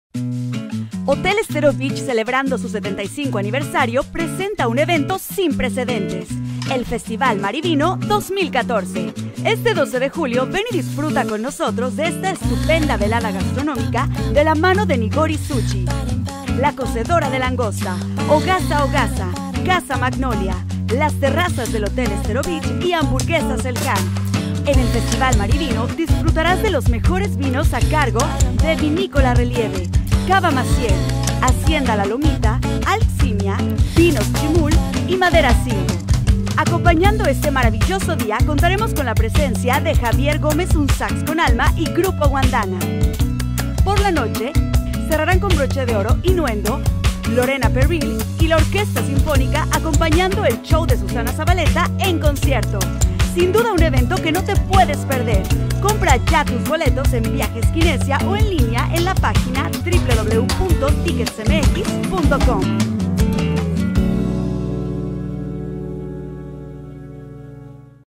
"Festival Mar & Vino 2014" en el Estero Beach Hotel/Resort
"Festival Mar & Vino 2014" a realizarse en las instalaciones del Estero Beach Hotel/Resort el 12 de julio del 2014 en el marco de su 75 aniversario.